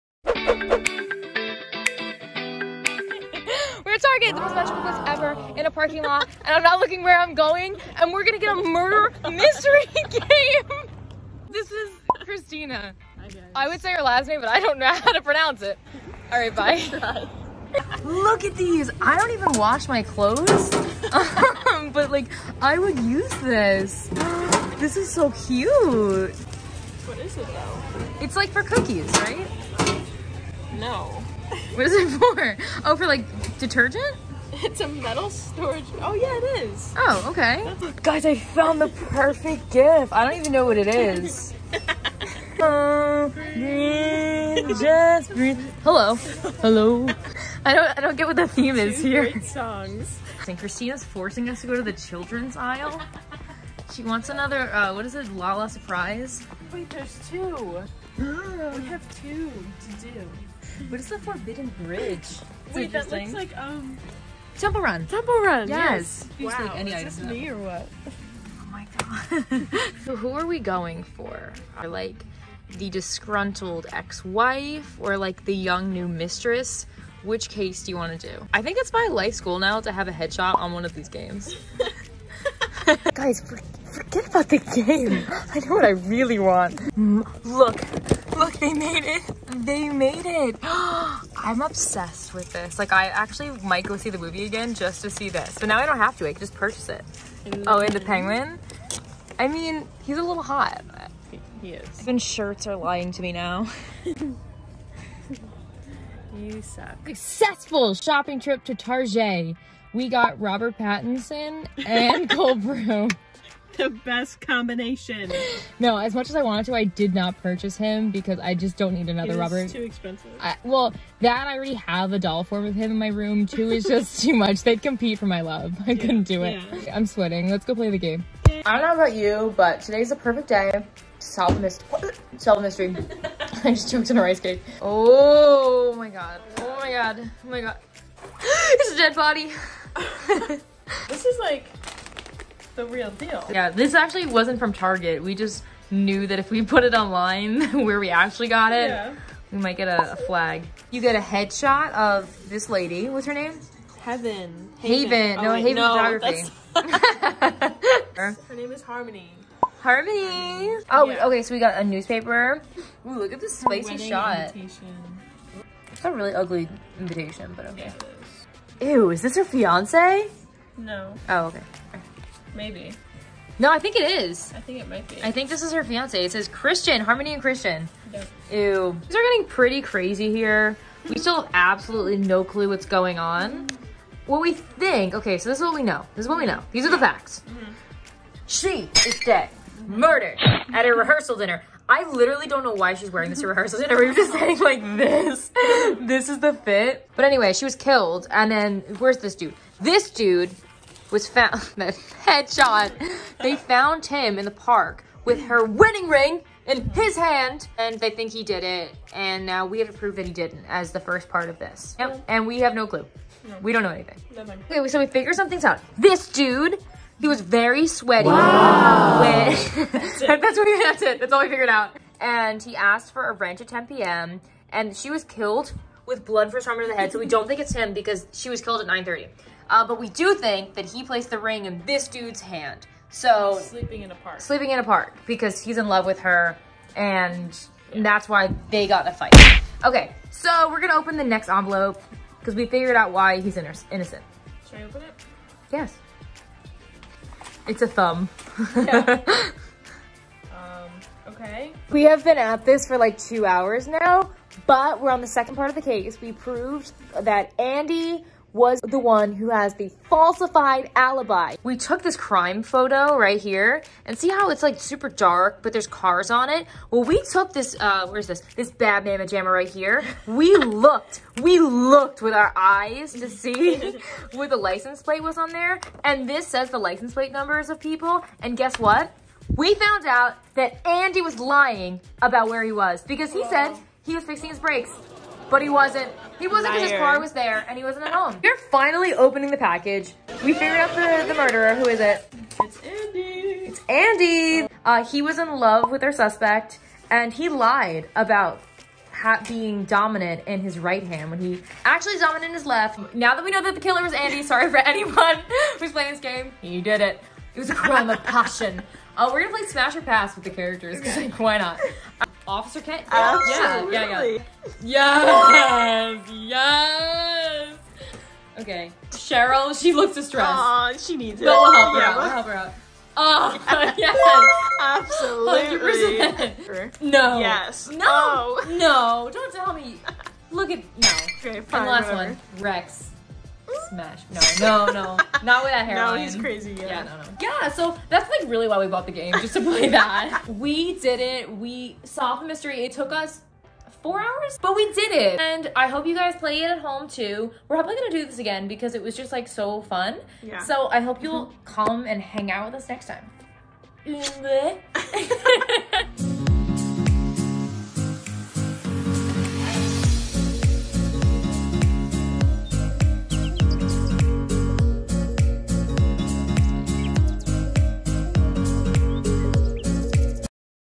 I found the BEST game at Target VLOG
Watch as Christina and I embark on a perilous journey to target. Together we face Robert Pattinson, questionable merchandise and above all else danger. Join us as we play Unsolved Case Files. Will we get to the bottom of the mystery￼ or be forced to turn in our highly official (Definitely NOT from Target) Detective badges?